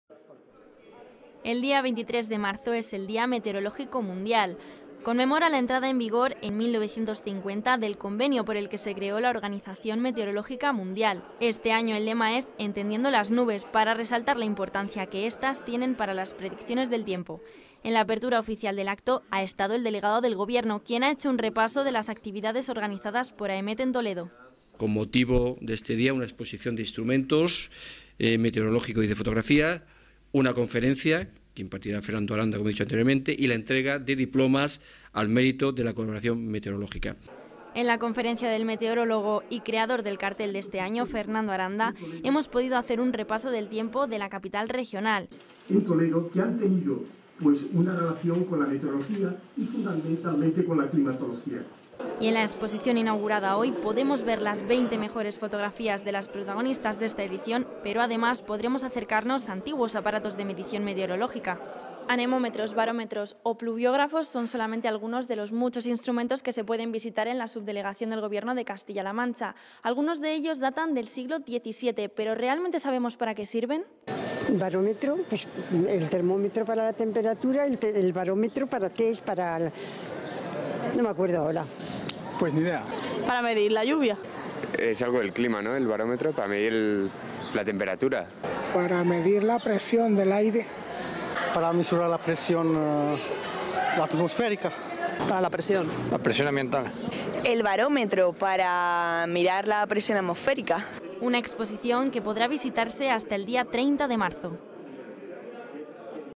DÍA METEOROLÓGICO MUNDIAL
El 23 de marzo se conmemora la entrada en vigor, en 1950, del convenio por el que se creó la Organización Meteorológica Mundial. Este año bajo el lema "Entendiendo las nubes". Para celebrar este día se han organizado actividades como la conferencia "Retazos de la historia, hidrología y climatología de Toledo" o la inauguración de la exposición de Instrumentos Meteorológicos, entre otras.